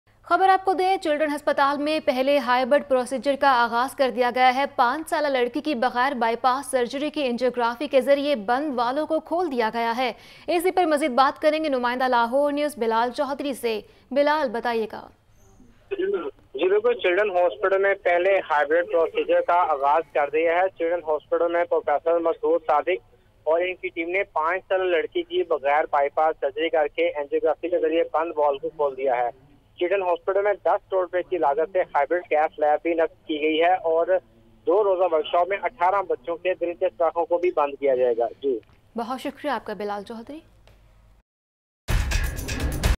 Pakistani Doctors Attempt Successful Heart Surgery Without Bypass | Breaking News - Lahore News HD
Pakistani Doctors Attempt Successful Heart Surgery Without Bypass